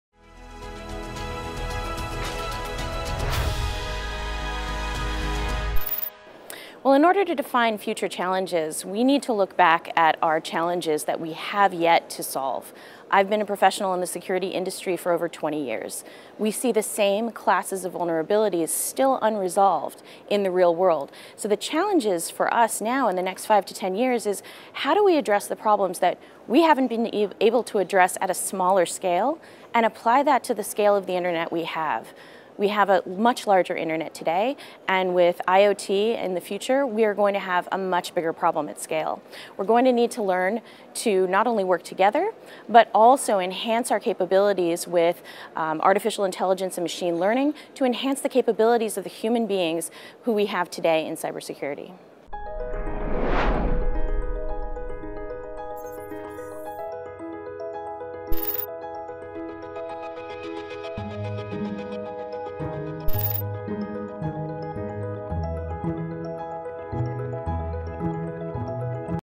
Luta Security CEO, Katie Moussouris on the future cybersecurity challenges | CYBERSEC 2018
4th European Cybersecurity Forum - CYBERSEC 2018
8-9 October 2018 - Krakow, Poland
The Quest for Cyber Trust
CYBERSEC TV Interview Series